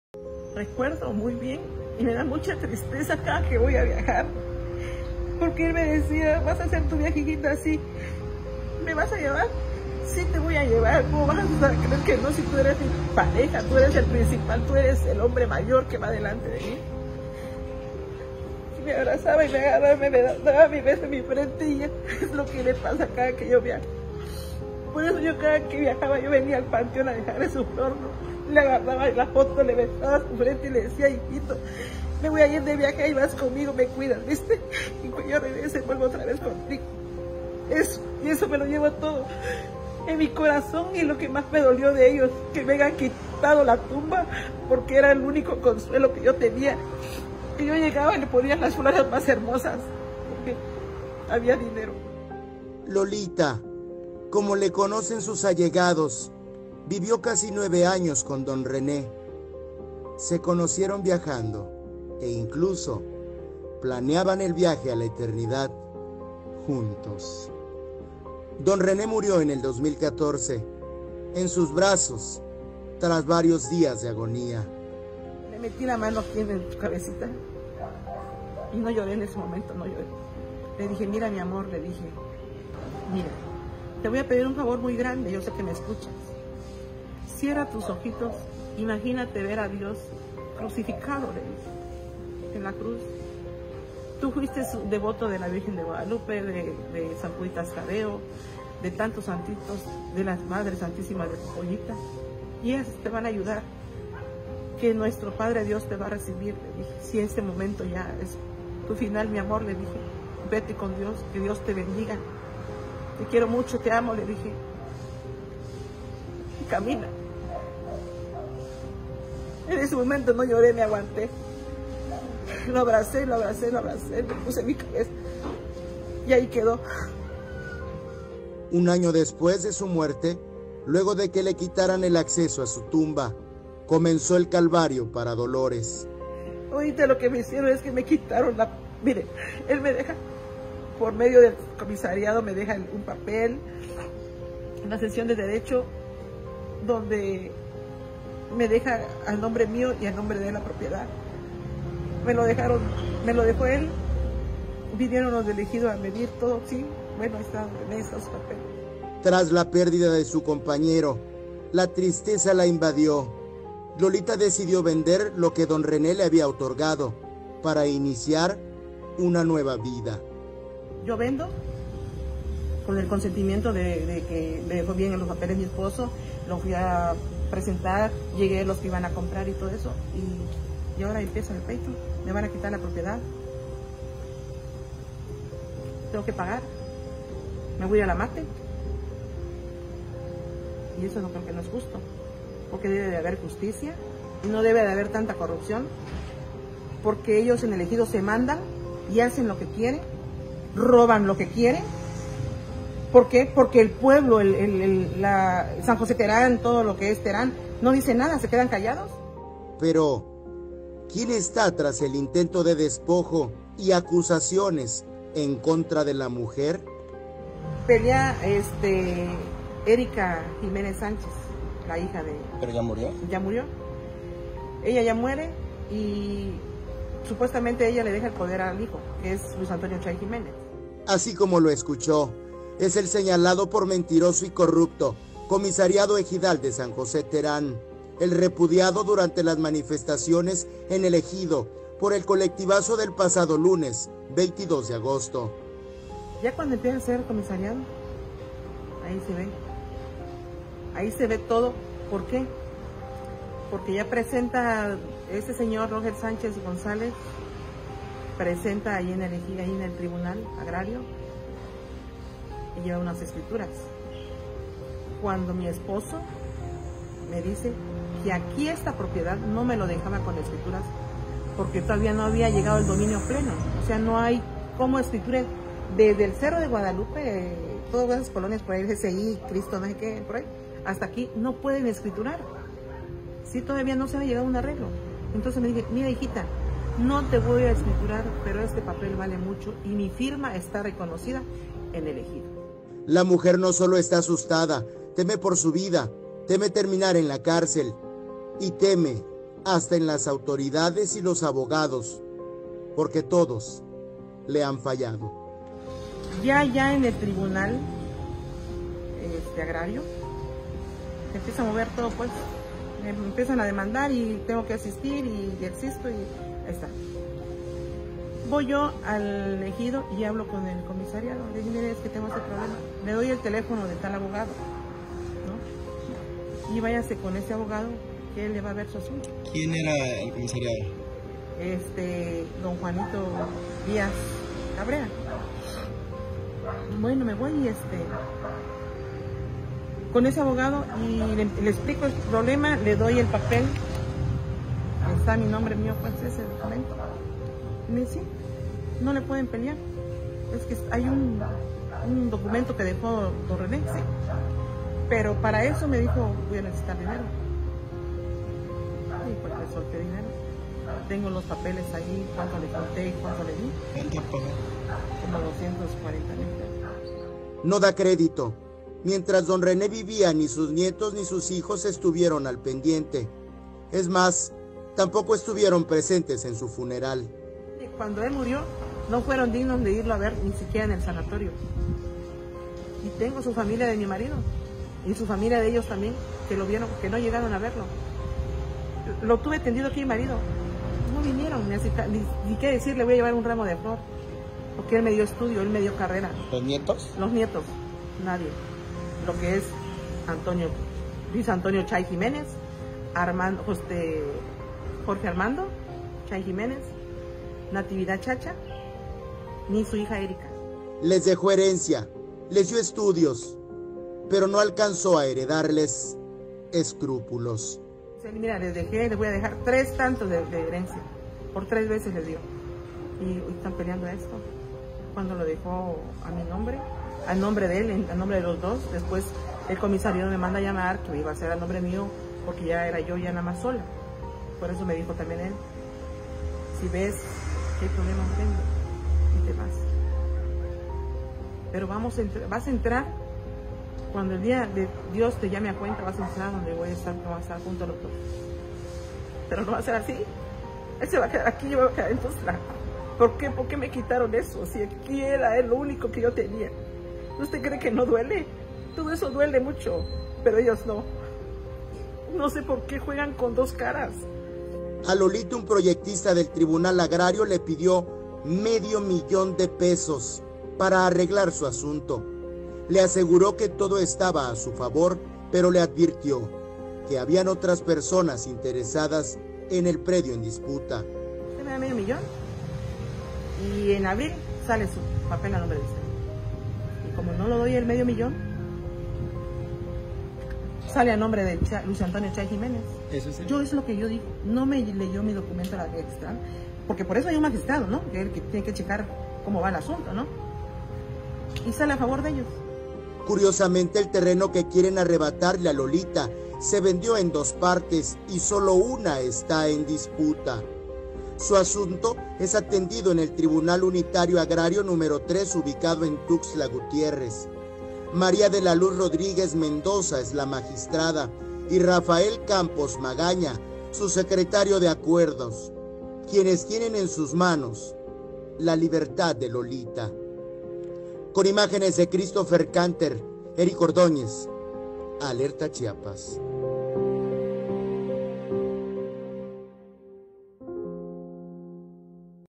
“Lolita” está de luto y a punto de terminar en la cárcel
El Comisariado de San José Terán arrasa con todo; con documentos falsos y tráfico de influencias, Antonio Chay, quiere apoderarse de lo que su abuelo cedió en vida a su viuda.